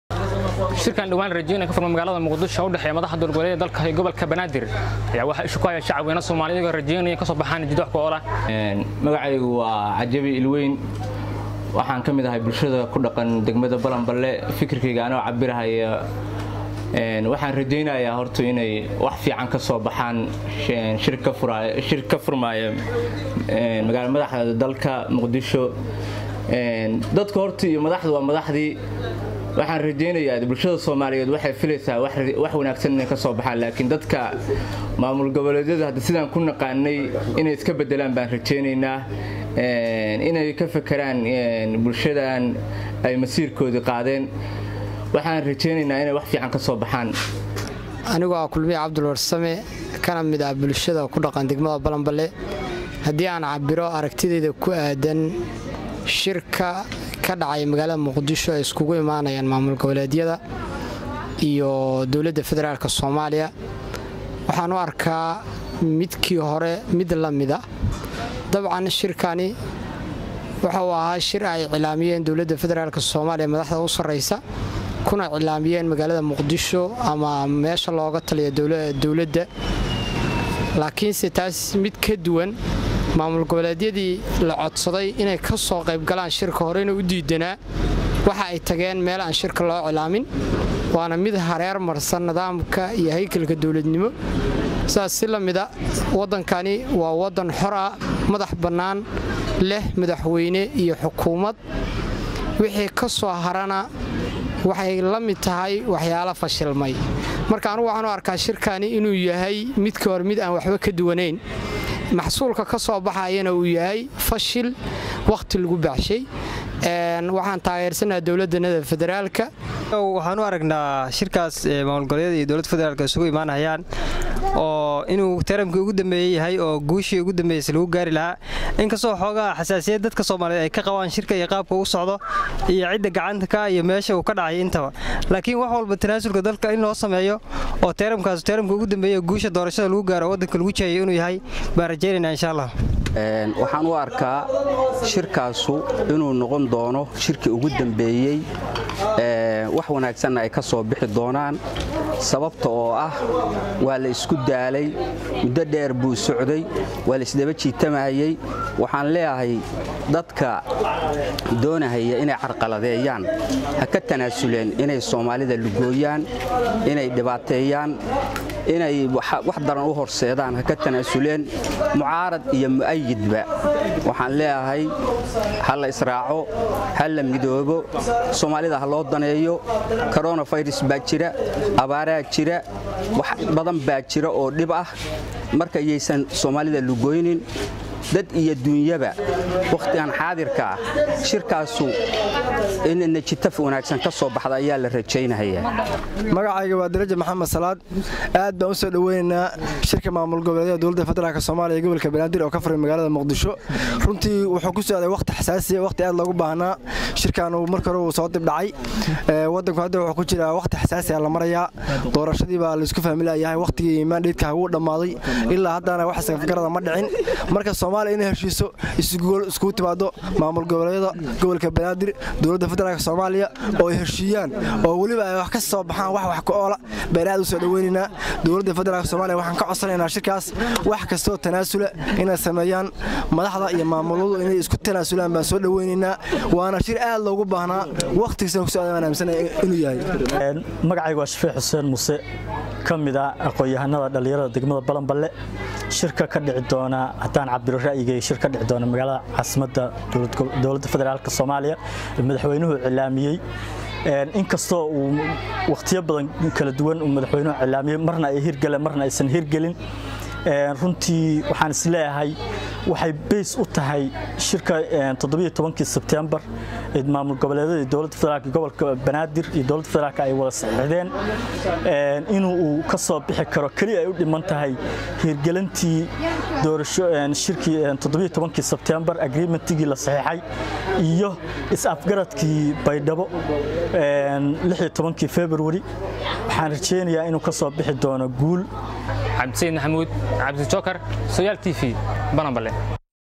Shacabka Dagmada Balanbale oo aragtidoda kadhibtay shirka DFS &Dowlad Goboledyada oga furmaya Muqdis
Shacabka Dagmada Balanbale oo aragtidoda kadhibtay shirka DFS &Dowlad Goboledyada oga furmaya Muqdisho